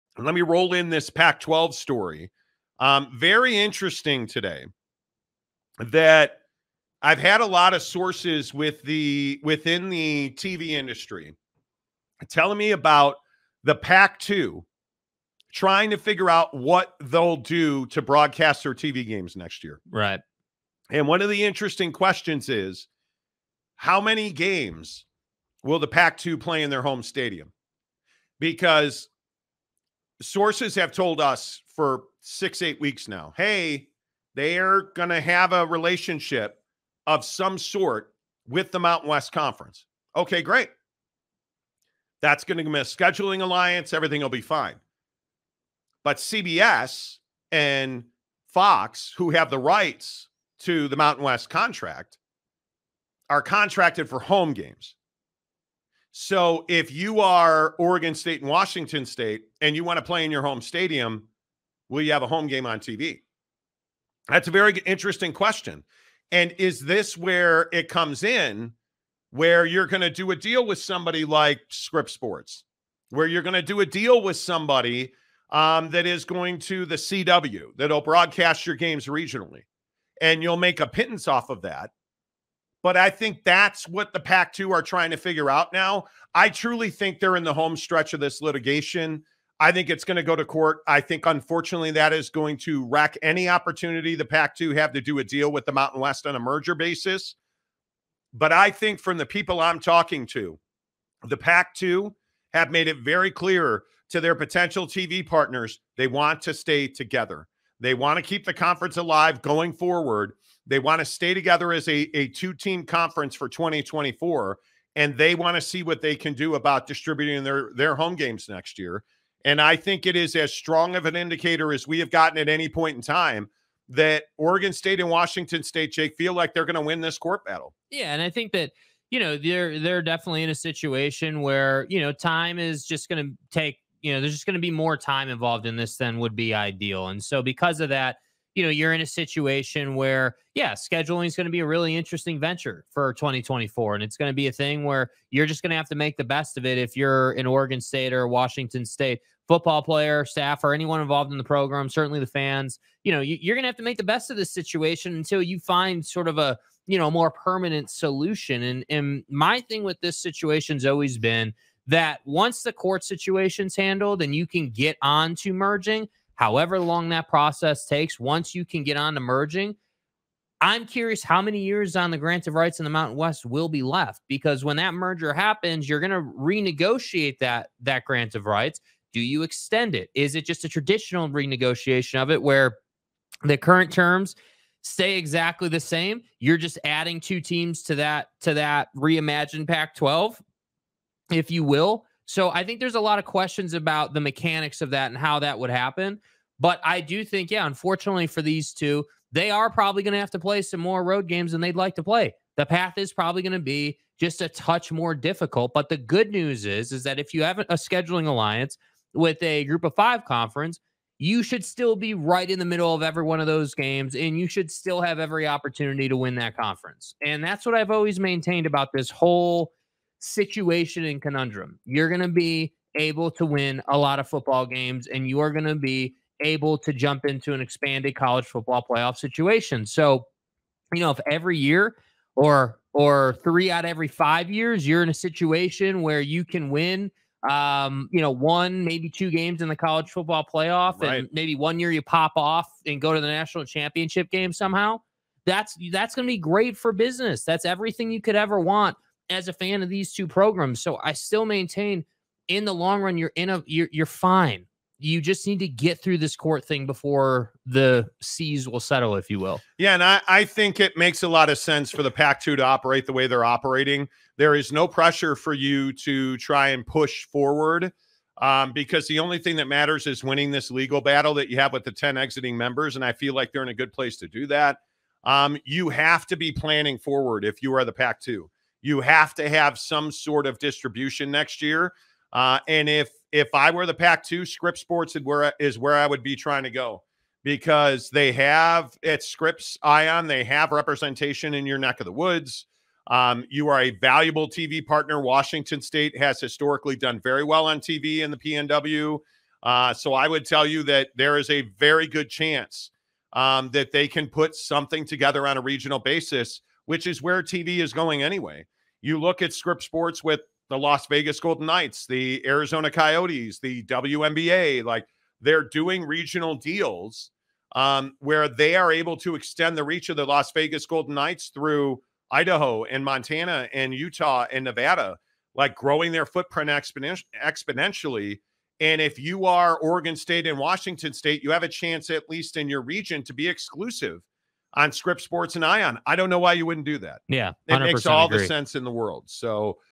What Will The PAC 12 ...PAC 2...Do In 2024?
The Monty Show has the latest details on what the "PAC 2" is going to do, and where the future of the PAC 12 is headed. Oregon State and Washington State have a home in the Mountain West Conference? Will the PAC 12 name survive? The Monty Show has the details!

BuckedUp Energy are giving you 6 FREE Buck Shots!